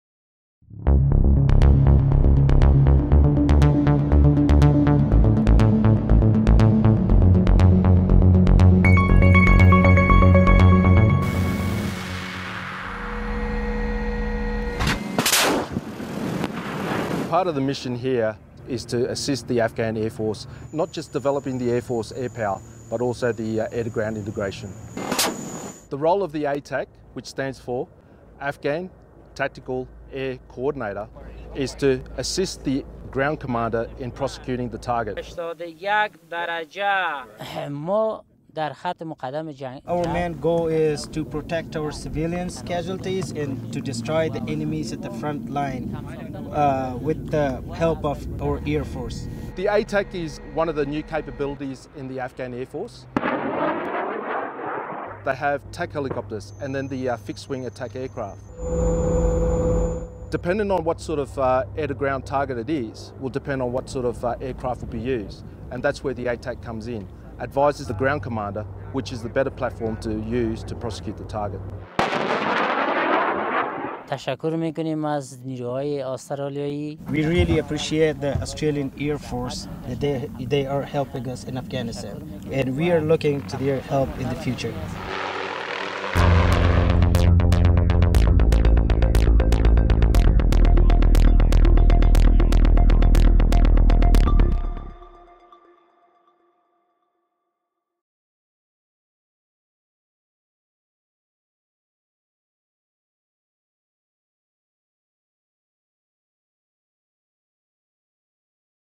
Afghan Tactical Air Coordinator Training
Ten Royal Australian Air Force members are embedded at Train, Advise, Assist Command – Air (TAAC-Air) providing mentoring support to their Afghan Air Force counterparts in Kabul, Afghanistan.